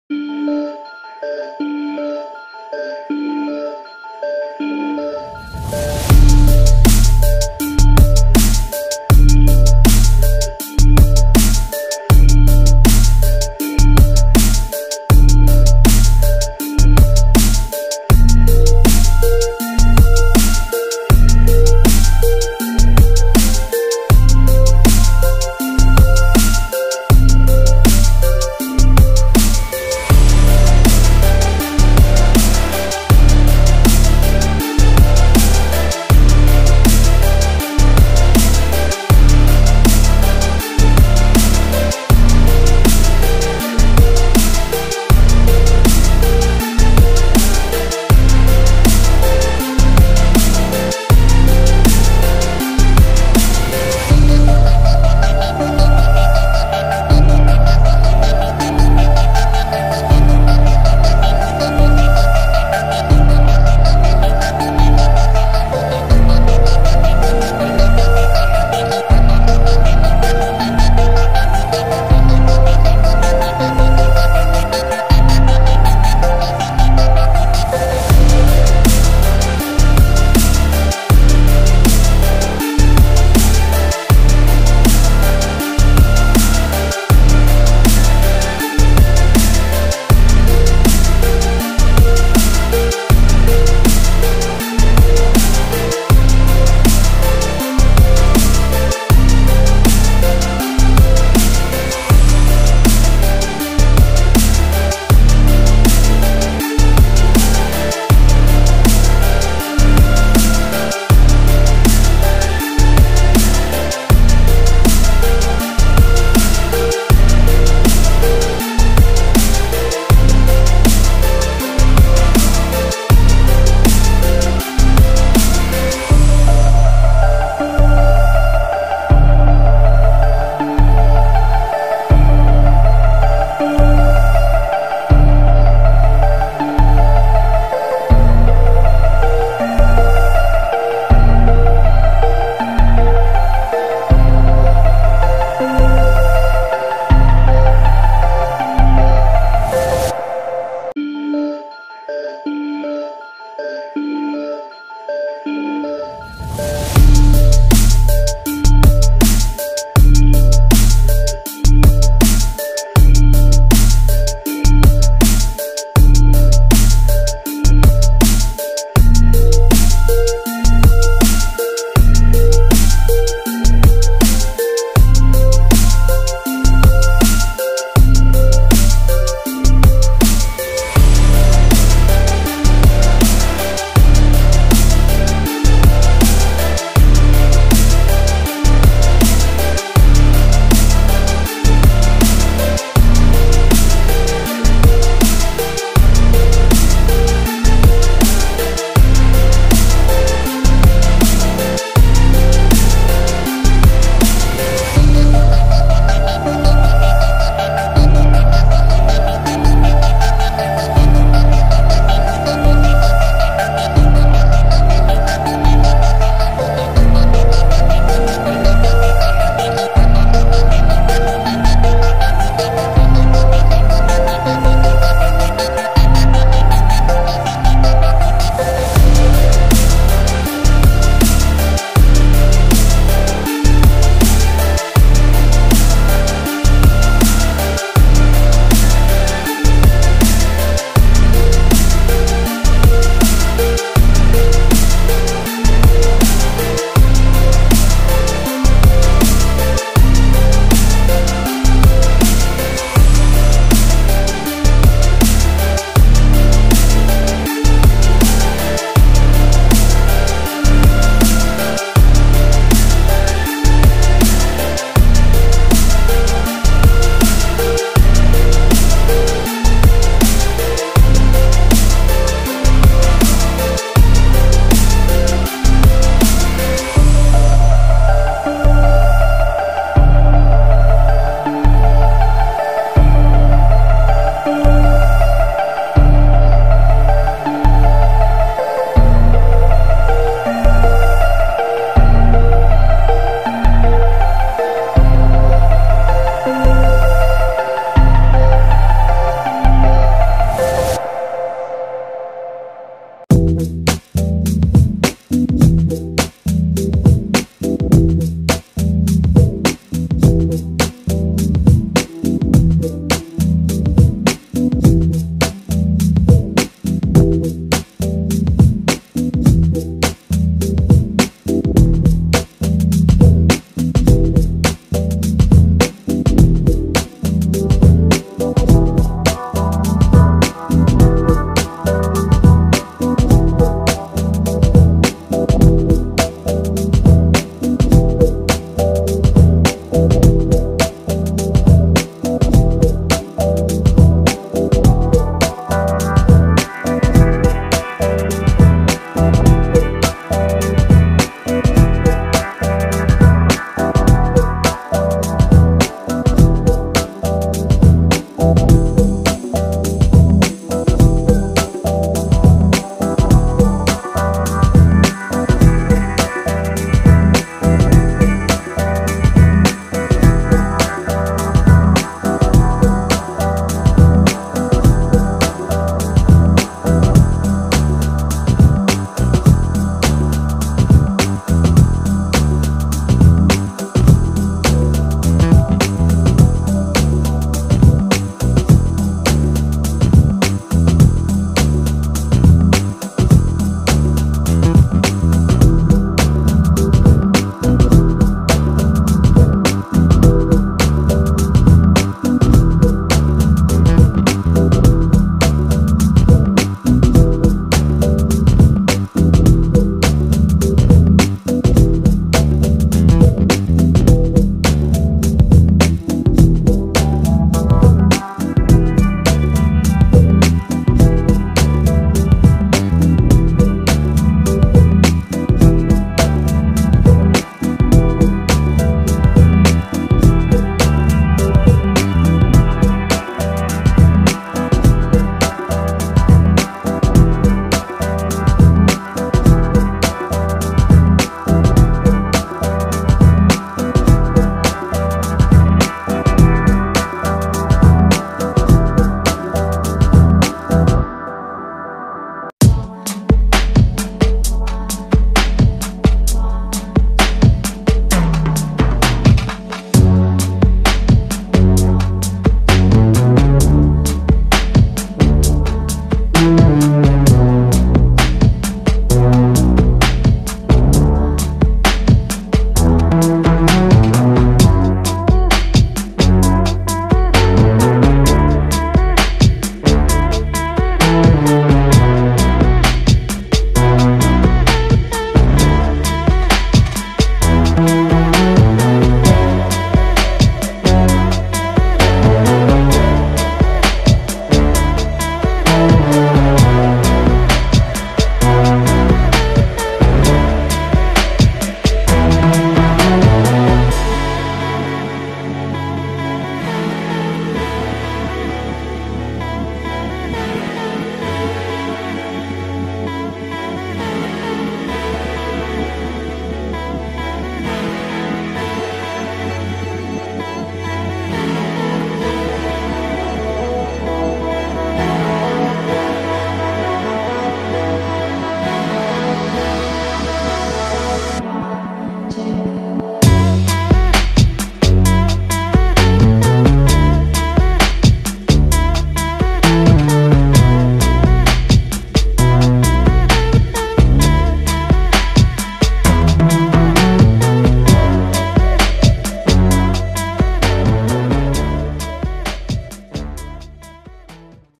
Rangkaian Pancing Kelabang - Teknik Dasaran
kali ini saya membuat rangkaian pancing kelabang teknik dasaran

rangkaian kelabang ini bisa untuk mancing nila menggunakan umpan cotton bud atau umpan cacing

dan rangkaian pancing kelabang ini juga bisa buat mancing ikan lainnya seperti mancing bandeng, mujaer, atau mancing dasaran di muara